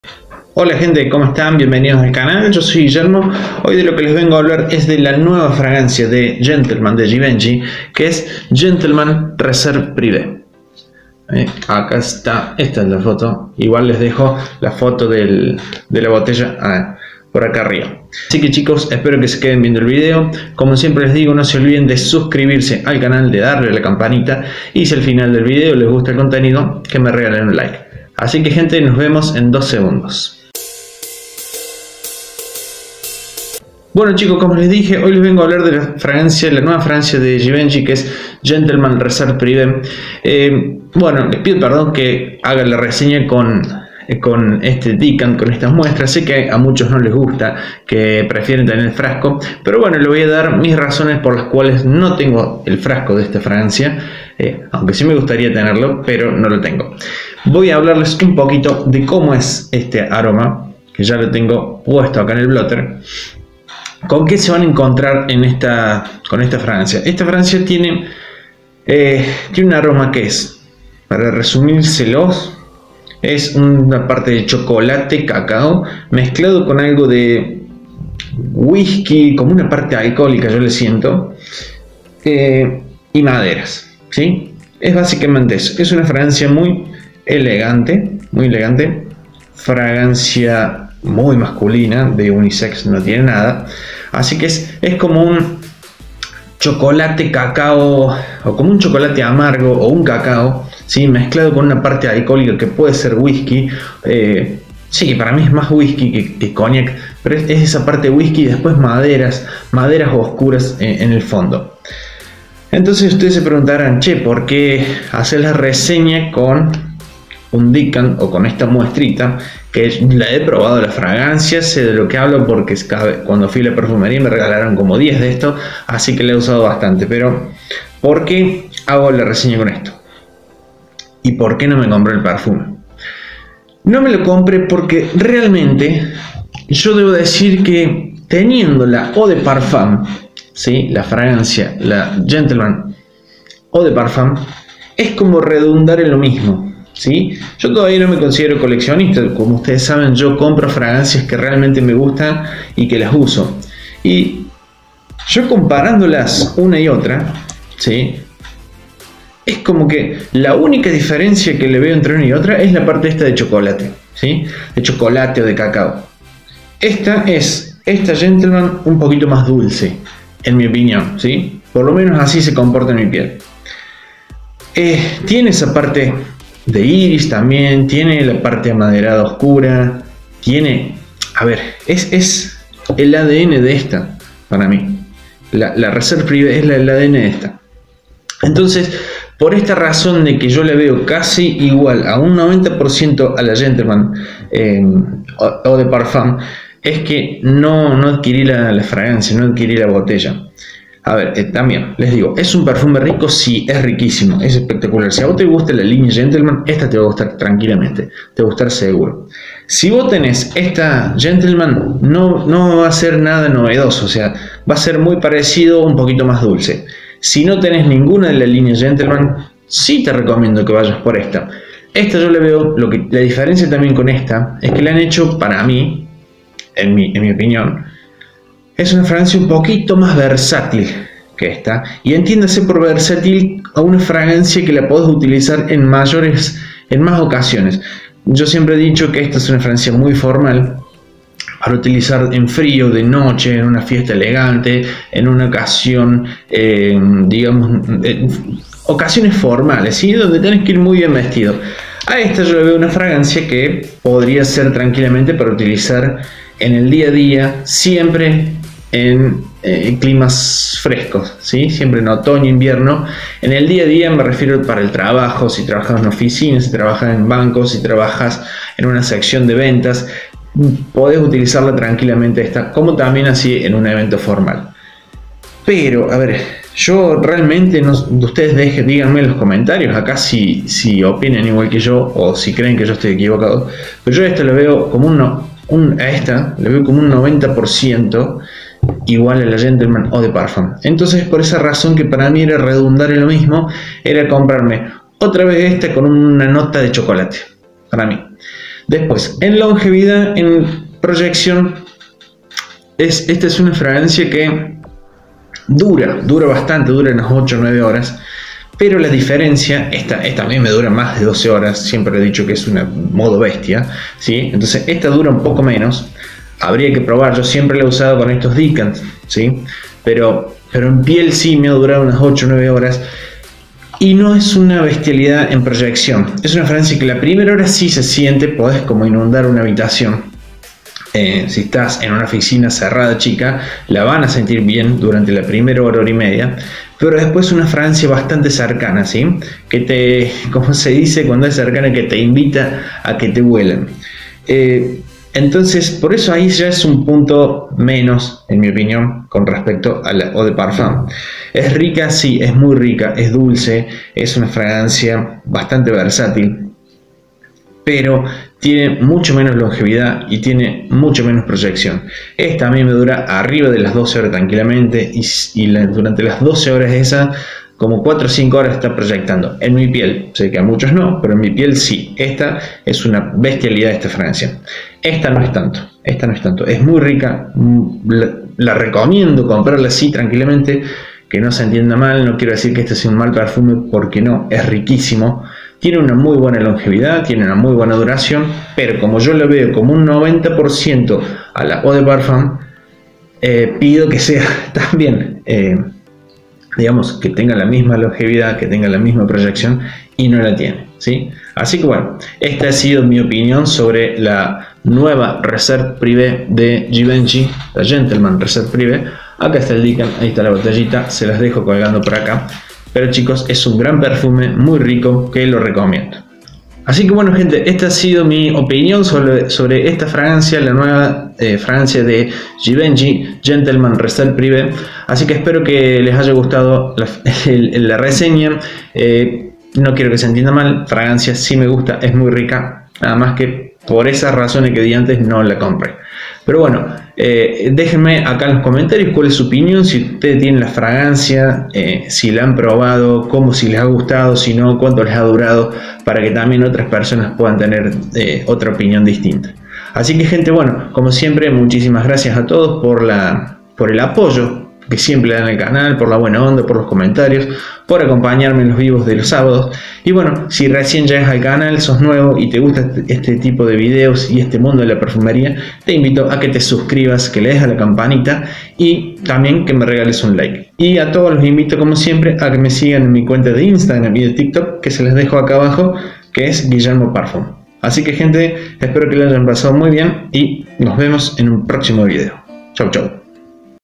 NUEVA!!! GENTLEMAN RESERVE PRIVEE - VALE LA PENA??? | GUILLERMO PARFUM
GENTLEMAN RESERVE PRIVEE, vale la pena??
en esta reseña les cuento mi opinon de esta Nueva Fragancia de givenchy, como huele, como es en comparacion con la Gentleman EDP
LOS INVITO QUE ME SIGAN EN INSTAGRAM Y TIK TOK